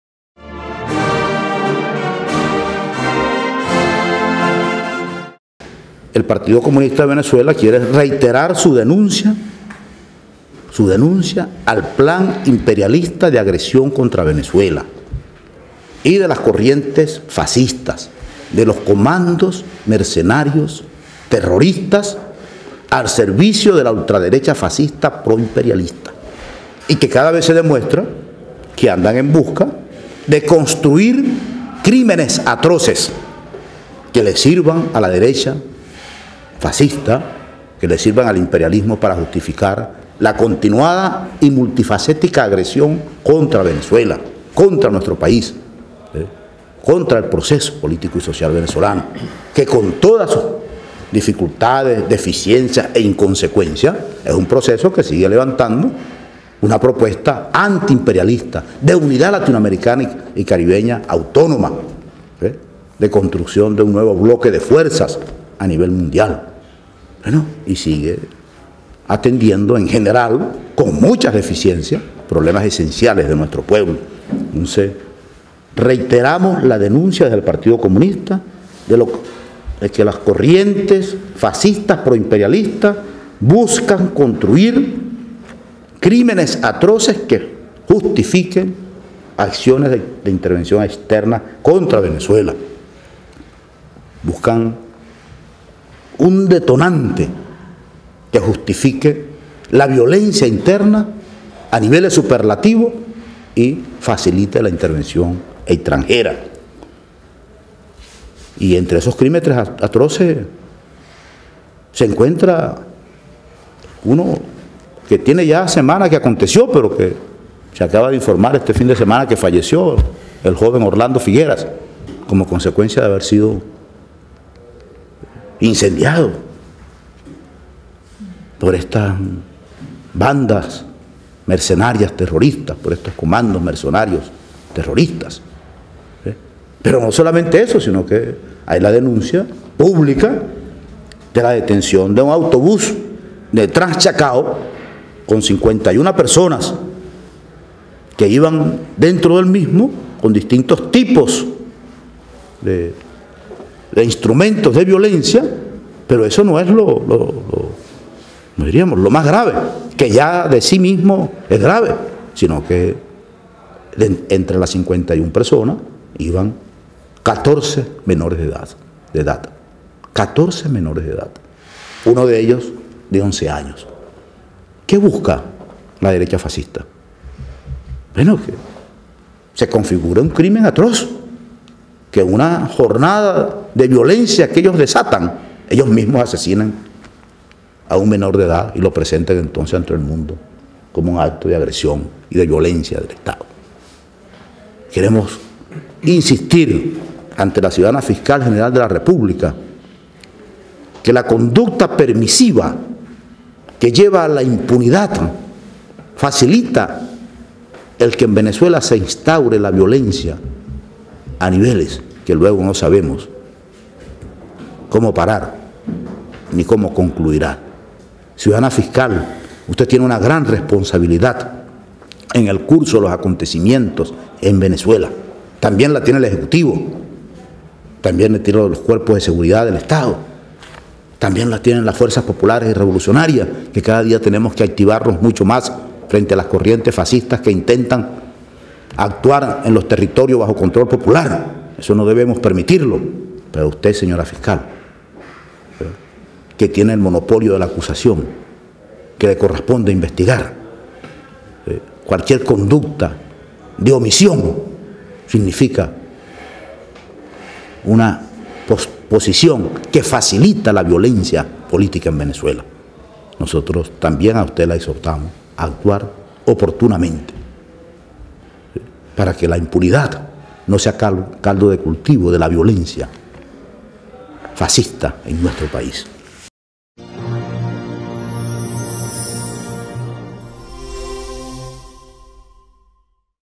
El PCV denuncia que la violencia fascista busca detonante para invasión extranjera
Caracas, 6 jun,. 2017, Tribuna Popular TP.- El Buró Político del Partido Comunista de Venezuela (PCV) reiteró la denuncia de los atroces crímenes que está cometiendo la oposición fascista en Venezuela cuyo objetivo es facilitar una intervensión extanjera en nuestro país y llamó a la Fiscal General, Luisa Ortega Díaz, a actuar diligentemente contra los autores materiales e intelectuales de estos crimenes, incluyendo la utilización de menores de edad.